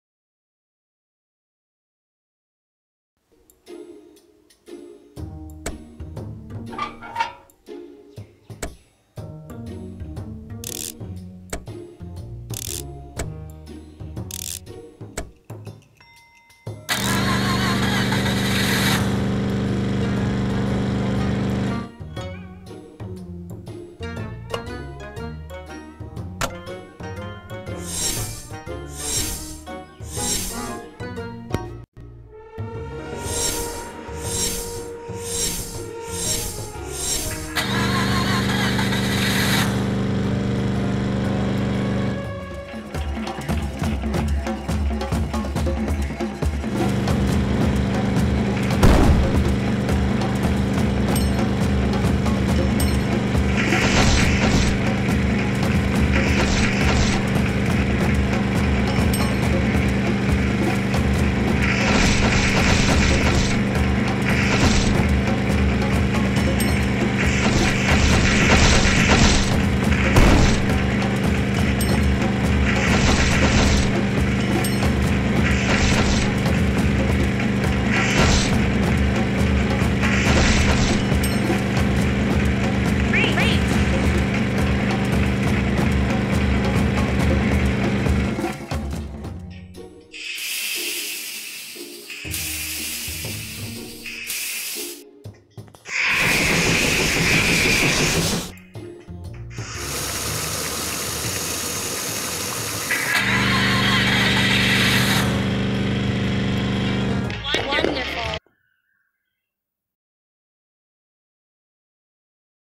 Car Race For Kids | Cartoon Car Race.
Ye Video Kids Ke liye Hai, Ye Ek Cartoon Car Race Ki Video Hai